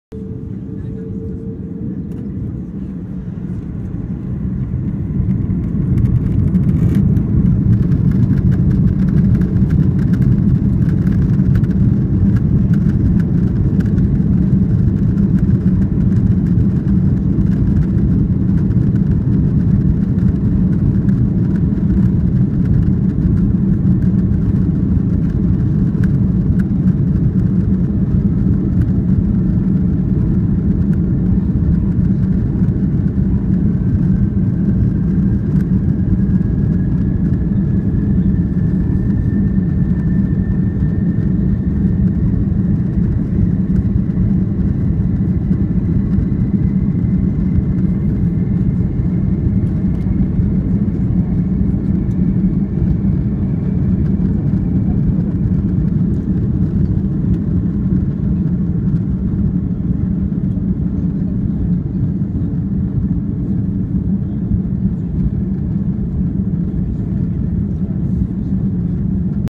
Take-off at Munich International Airport on a Lufthansa Airbus A340-600
Flight LH458 to San Francisco, CA, United States on February 13, 2014.